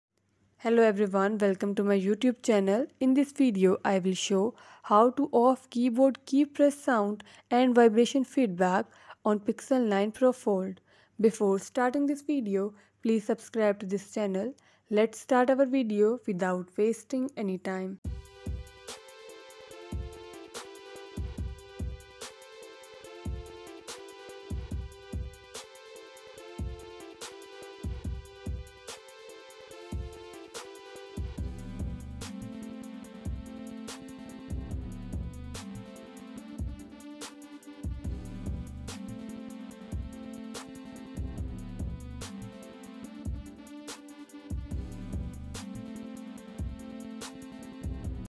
Google Pixel 9 Pro Fold Keyboard Settings || Turn ON/OFF Keyboard Sound
in this video you will learn Google Pixel 9 Pro Fold Keyboard Settings || Turn ON/OFF Keyboard Sound